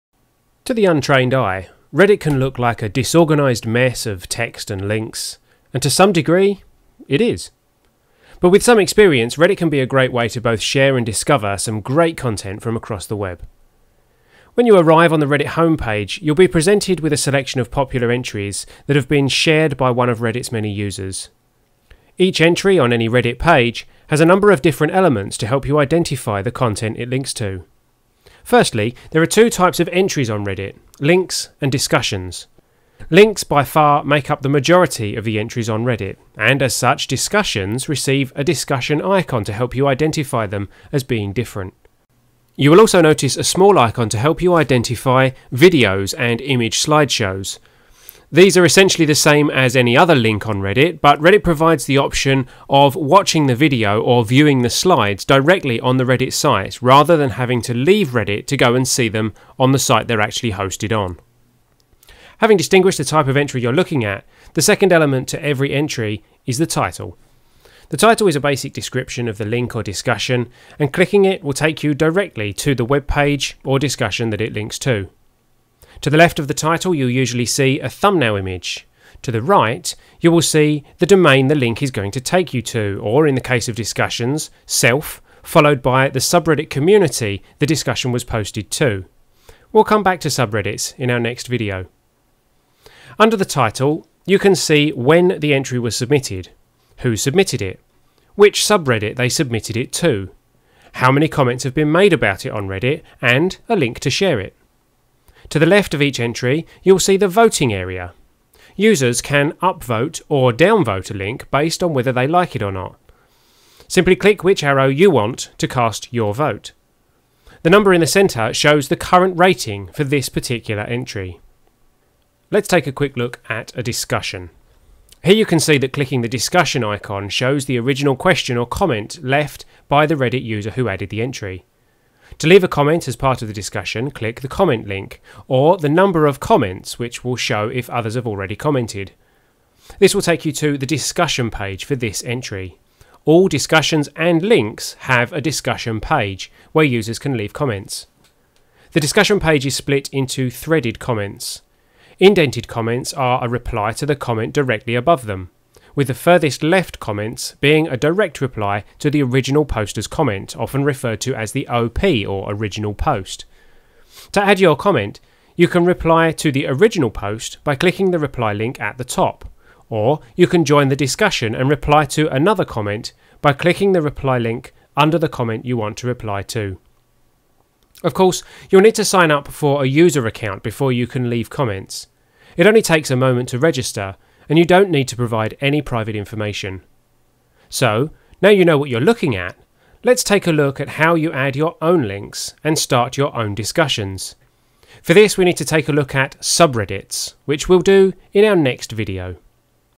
How To Use Reddit
Video one in our three part series: Learn the basics of the Reddit interface